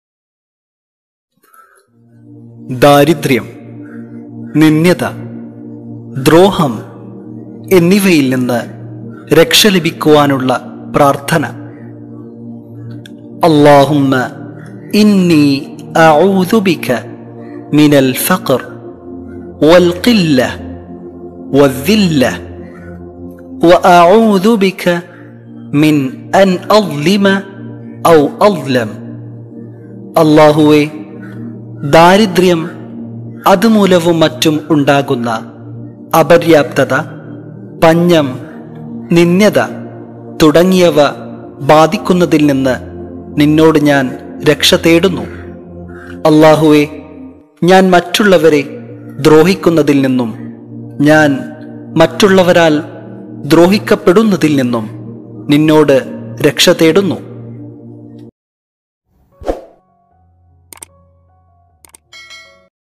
പ്രാർത്ഥന | short video | ദ്രോഹം, ദാരിദ്ര്യം, നിന്ദ്യത യിൽ നിന്ന് രക്ഷ നേടാൻ
*🤲🏻اللّهُمَّ إنِّي أَعُوذُ بِكَ مِنَ الْفَقْرِ، وَالْقِلَّةِ، وَالذِّلَّةِ، وَأَعُوذُ بِكَ مِنْ أَنْ أَظْلِمَ أَوْ أُظْلَمَ*

_✨അല്ലാഹുവേ! ദാരിദ്ര്യം (അതുമൂലവും മറ്റും ഉണ്ടാകുന്ന) അപര്യാപ്തത, പഞ്ഞം, നിന്ദ്യത തുടങ്ങിയവ ബാധിക്കുന്നതിൽ നിന്ന് നിന്നോട് ഞാൻ രക്ഷതേടുന്നു.അല്ലാഹുവേ! (ഞാൻ മറ്റുള്ളവരെ) ദ്രോഹിക്കുന്നതിൽ നിന്നും; (ഞാൻ മറ്റുള്ളവരാൽ) ദ്രോഹിക്കപ്പെടുന്നതിൽ നിന്നും നിന്നോട് രക്ഷതേടുന്നു_

#🔍Ref: أأخرجه أبو داود، كتاب الوتر، باب في الاستعاذة، برقم 1544،والنسائي، كتاب الاستعاذة، باب الاستعاذة من الذلة، برقم 5475، والنسائي في الكبرى